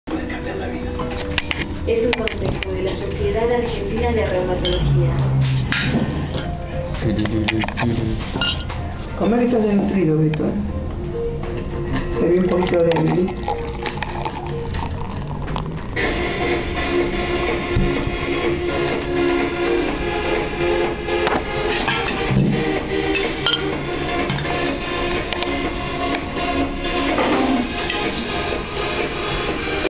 Cenando Pizzas En Casa Y Tomando Una Rica Cerveza.
A FULL EN CASA COMIENDO UNAS DELICIOSAS PIZZAS Y TOMANDO UAN RICA CERVEZA.